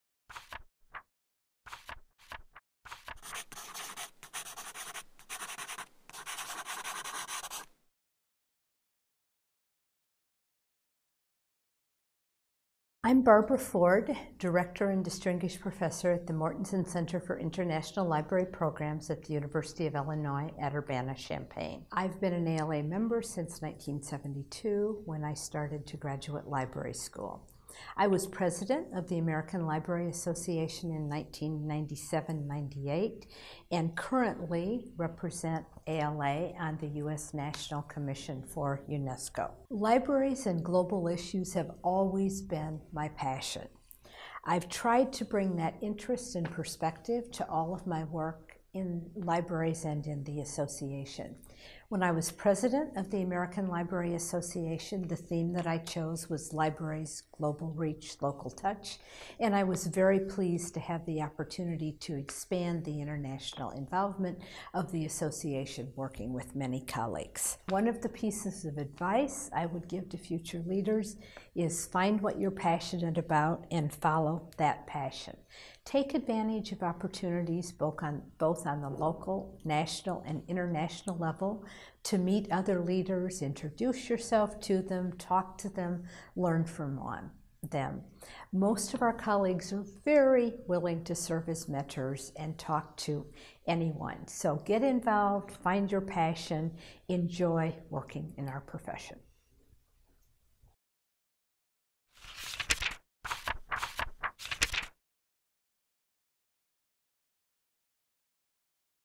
Empowering Diverse Voices-Barbara J. Ford
Barbara J. Ford, Director and Distinguished Professor, Mortenson Center for International Library Programs, University of Illinois at Urbana-Champaign Library

Barbara has worked in academic, public, and special libraries in Illinois, Texas, and Virginia and served as president of the American Library Association in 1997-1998 where her theme was " Libraries: Global Reach -- Local Touch". At the Mortenson Center she has the pleasure of working with librarians from around the world in a variety of continuing education and partnership projects.